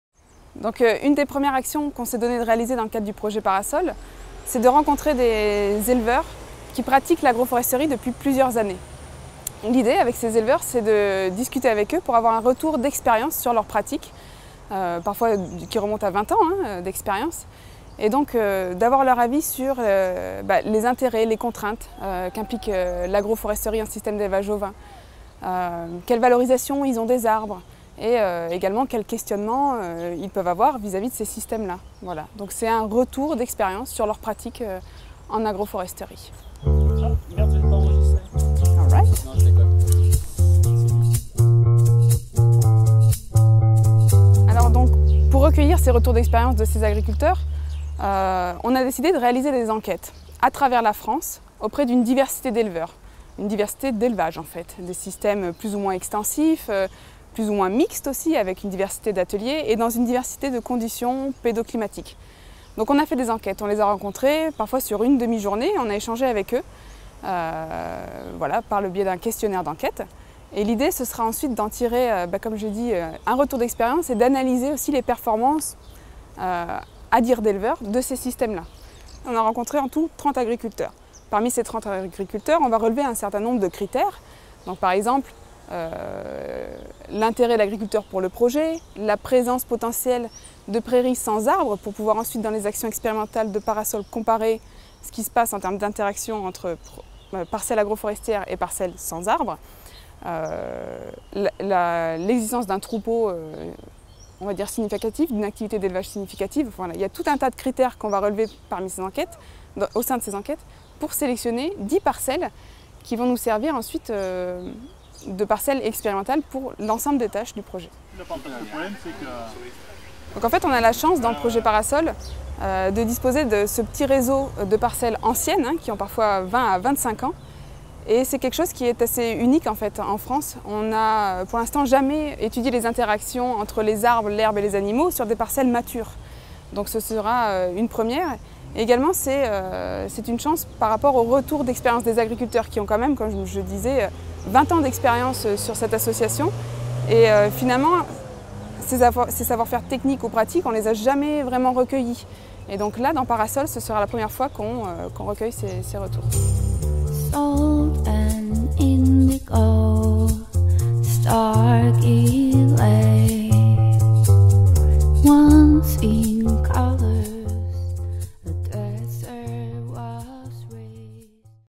Les performances de l'agroforesterie : retour des éleveurs. Action 1 PARASOL
PARASOL est un projet coordonné par la scop AGROOF qui à pour objectifs d'étudier les impacts bioclimatiques des arbres dans les systèmes d'élevage ovins, dans le contexte de changement climatique. L'action 1 du projet est une état des lieux de la diversité des système agroforestiers d'élevage en France et de leur performance. Explication par camille BERAL coordinatrice du projet et responsable de l'action 1.